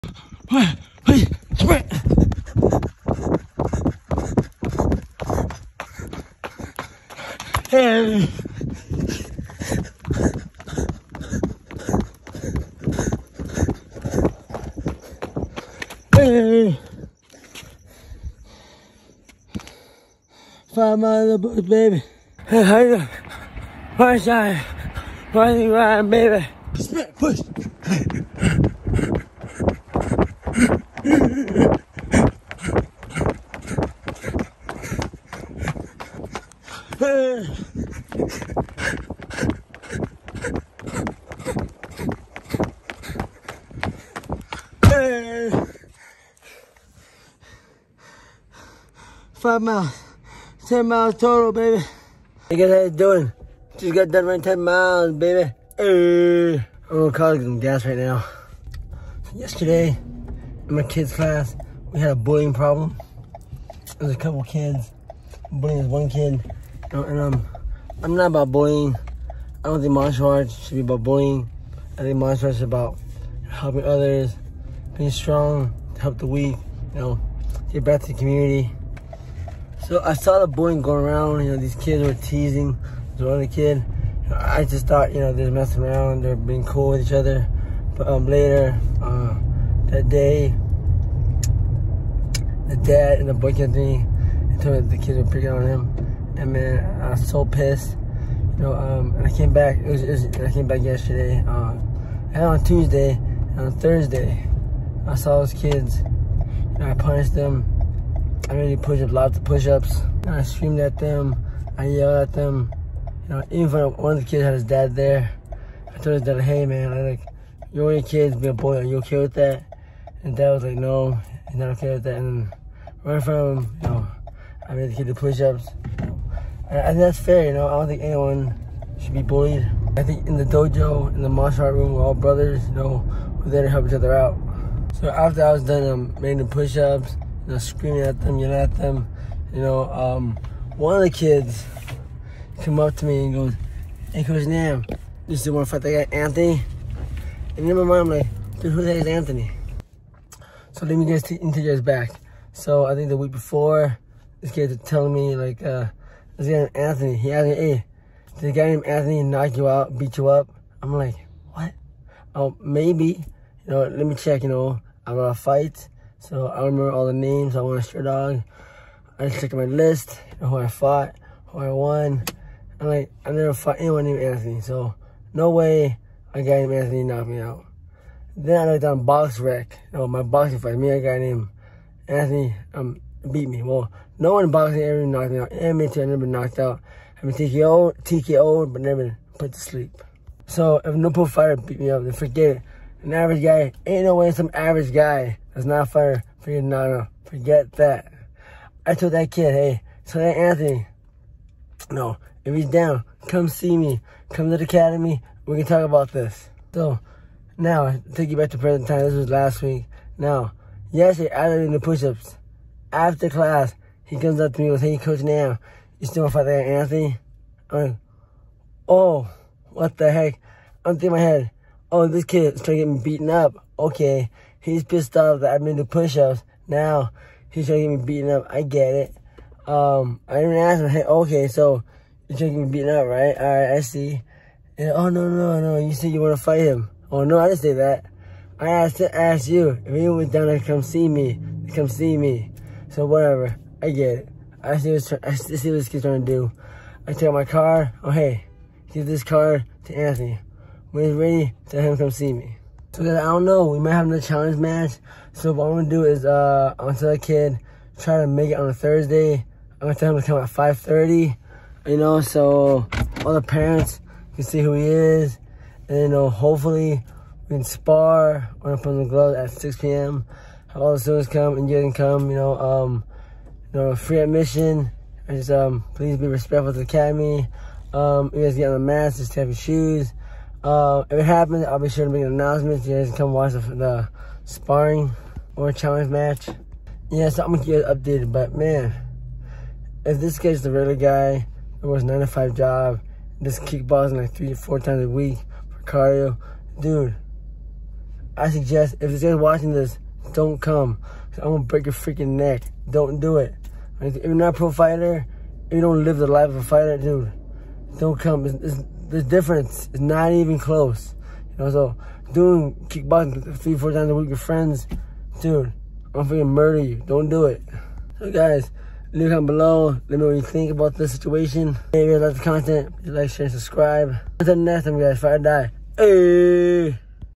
A New Dojo Challenger?
Some kids were bullying this one kid in my kids so i got mad and them do a lot of push ups. One of the wants revenge and ask if i want to fight his friend . Watch the video for full details